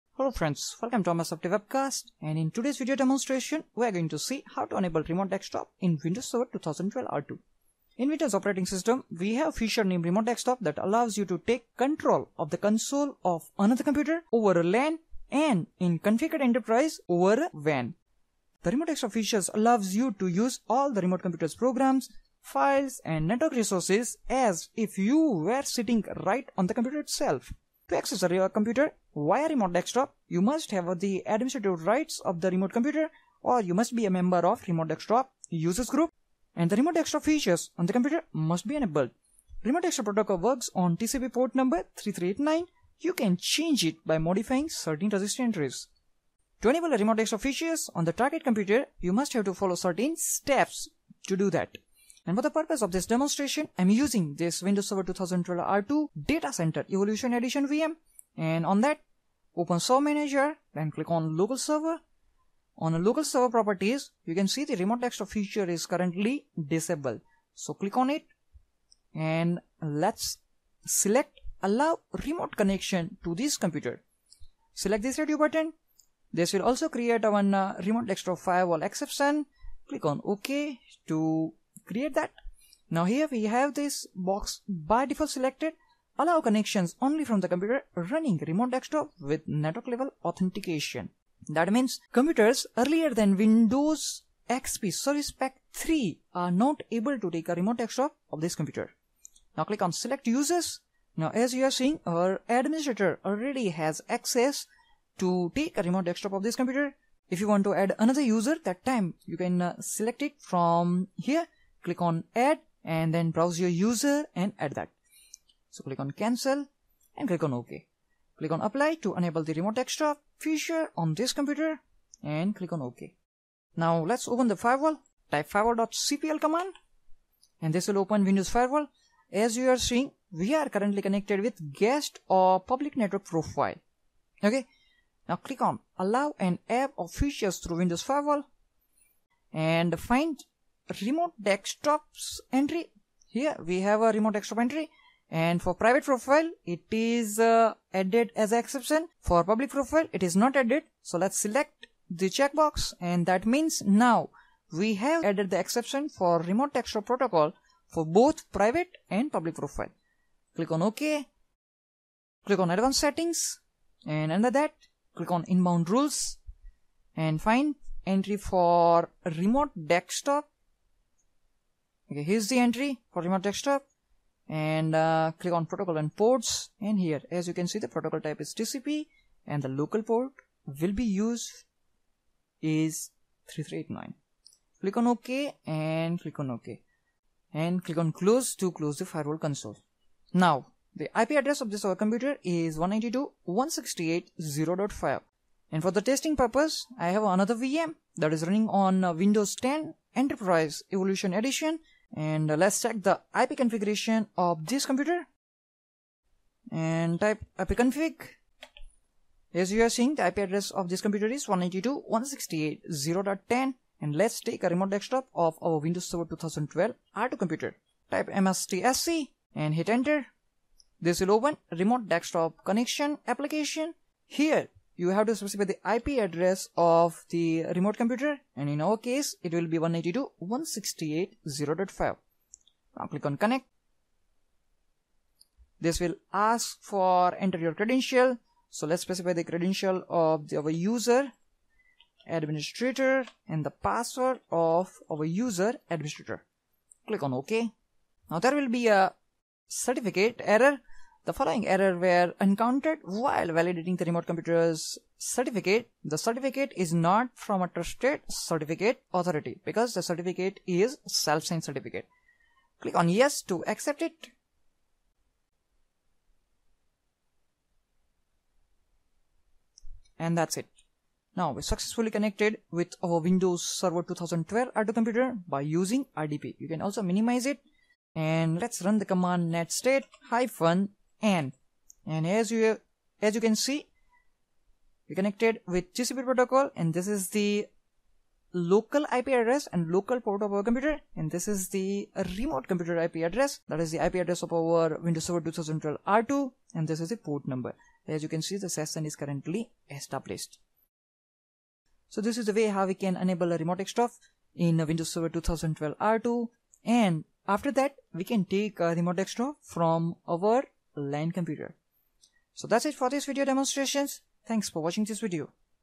How to Enable Remote Desktop in Windows Server 2012 R2
In this video demonstration we will see how to enable remote desktop feature (RDP) in Windows Server 2012 R2, as well as we will see how to connect Windows Server 2012 R2 from another computer using Remote Desktop Protocol (RDP).